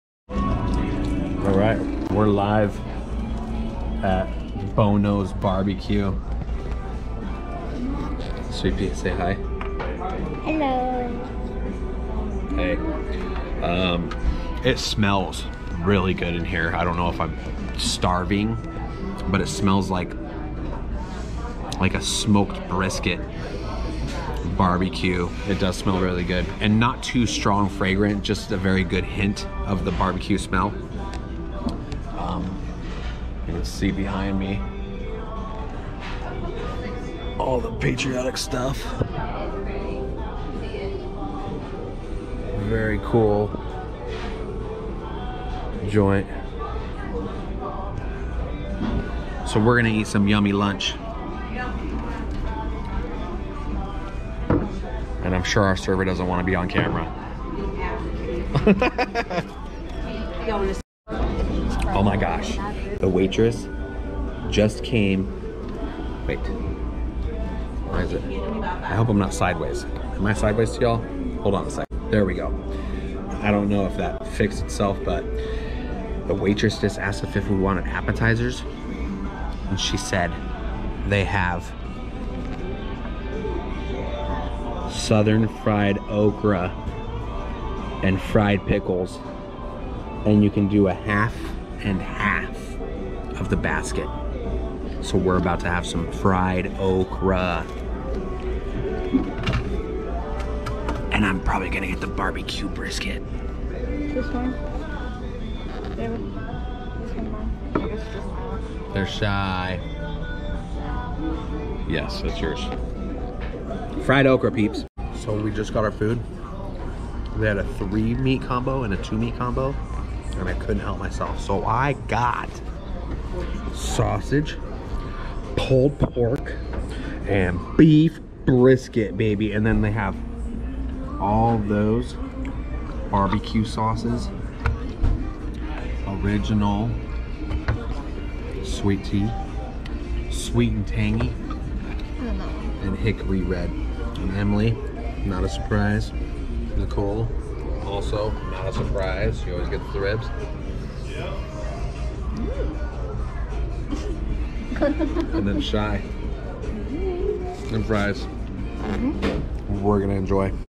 Bonos BBQ in Jacksonville Florida
We went to Bonos BBQ in Jacksonville Florida! Awesome BBQ! I'm sure it's not the best, but it was definitely delicious!!!